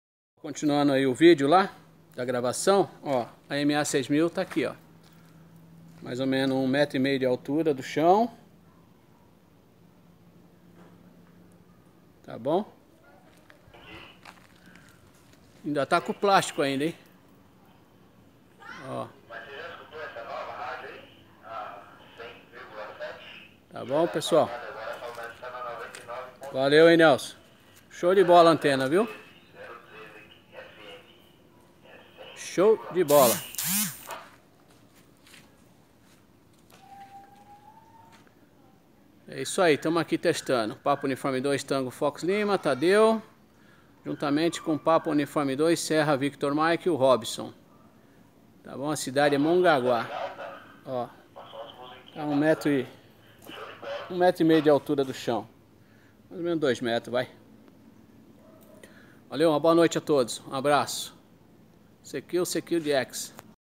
Tadeu de Mongagua - SP Aferindo a MA6000 - Parte 2 - Valentina Comercio
A Segunda Parte da Aferição da Antena MA6000 Voyager pelo nosso amigo Tadeu de Mongagua-SP Adquirida na Valentina Comercio e Importação